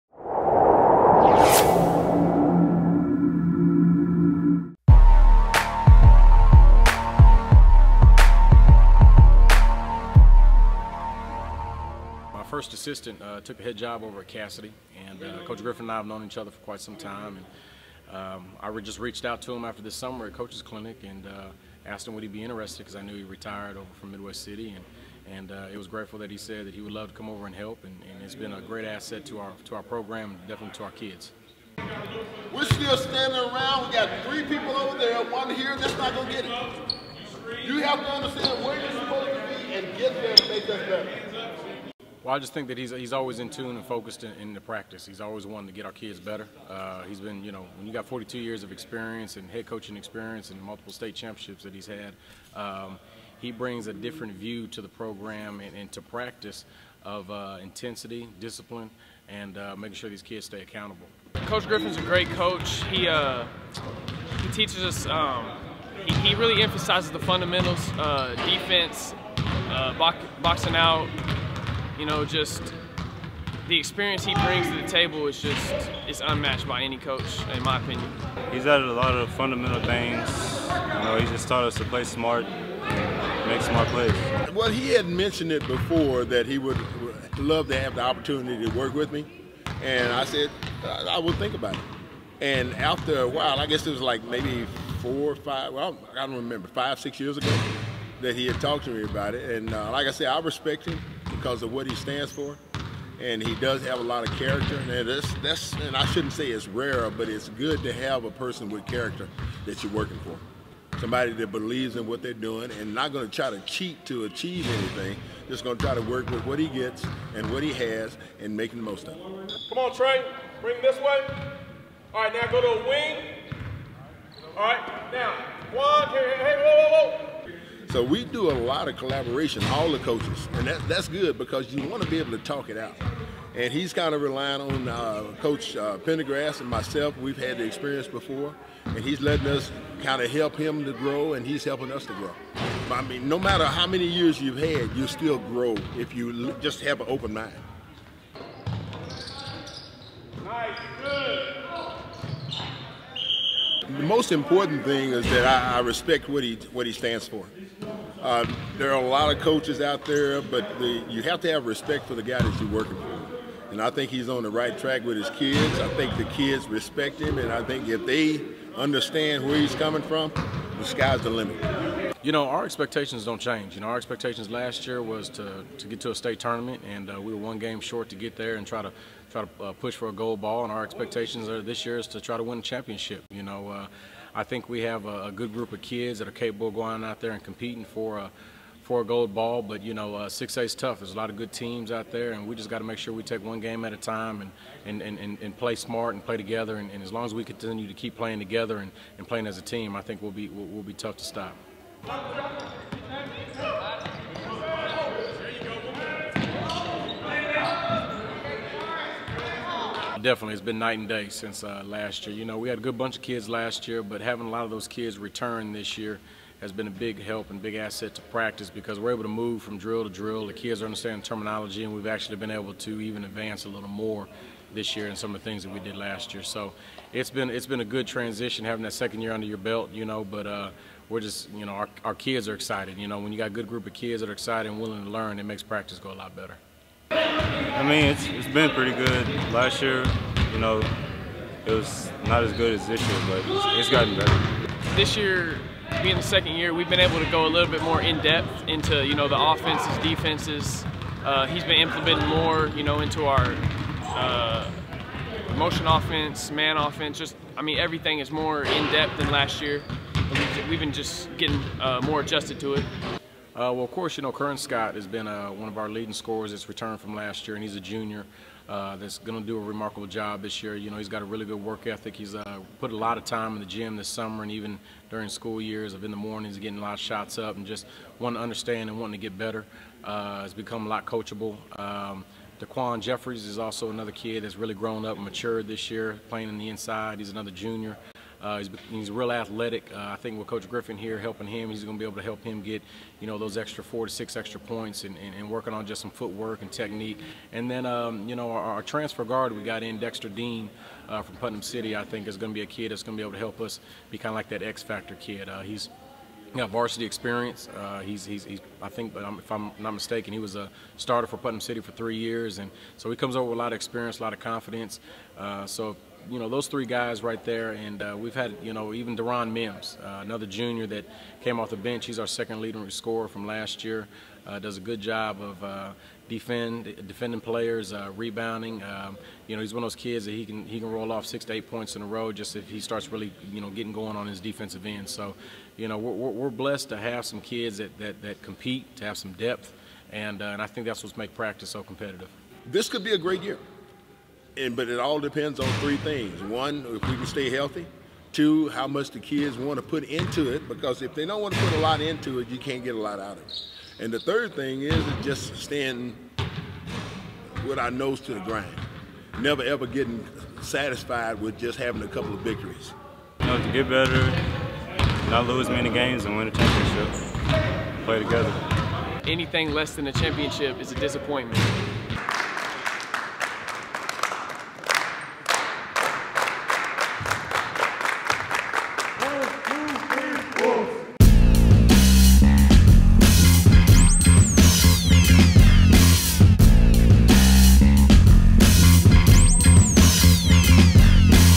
OK Hoops Behind The Scenes: Edmond Santa Fe
Coach Lenny Hatchett and the Edmond Santa Fe Wolves are currently ranked #3 in 6A. OK Hoops paid a visit to their practice recently to give you a behind the scenes look. As good as the Wolves could be on the court with Curran Scott, DaQuan Jefferies and company, they could be even better on the bench. Hatchett added Tommy Griffin to his staff this off season giving them an edge on the sidelines that few can match. Check out the exclusive interview and video with Hatchett, Griffin, Scott and Jefferies.